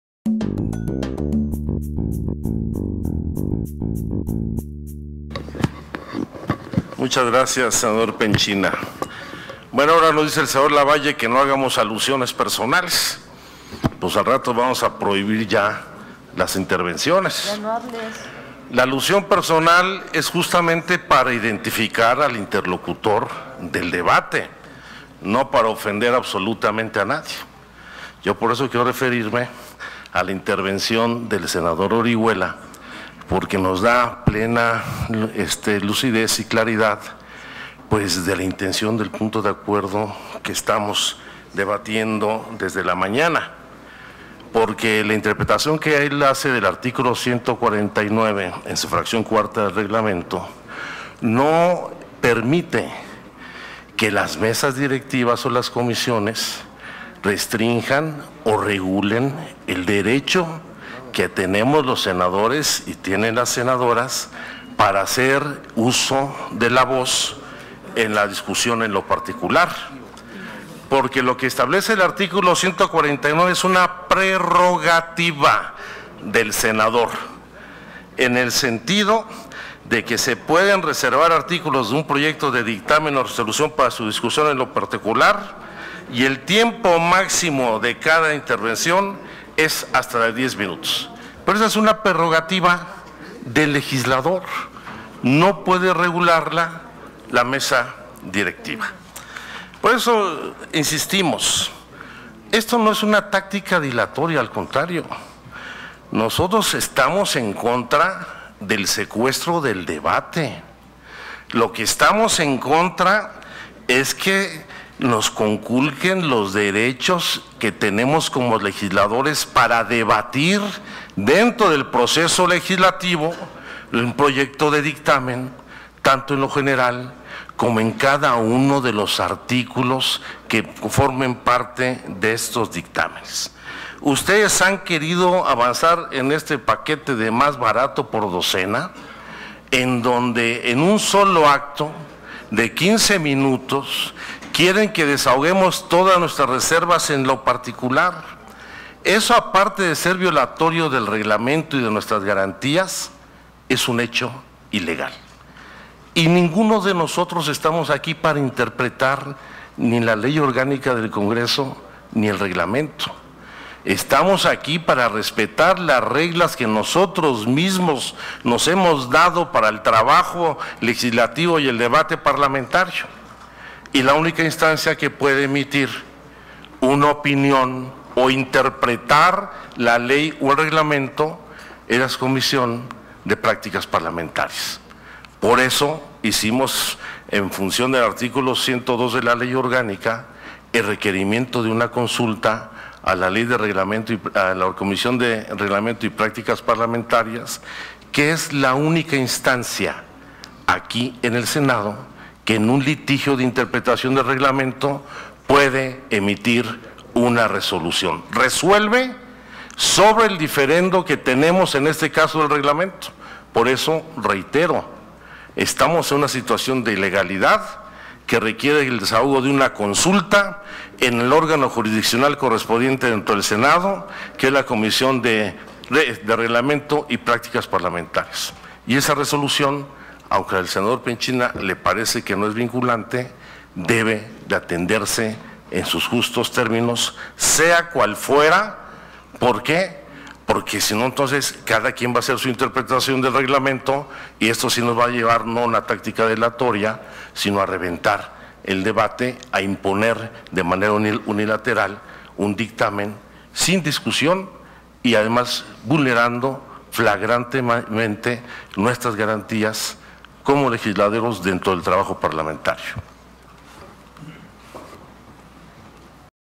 10/06/2014 Sen. Alejandro Encinas - Cuarta participación
Cuarta participación del senador Alejandro Encinas Rodríguez durante la reunión de trabajo de las comisiones unidas de Energía y de Estudios Legislativos Primera.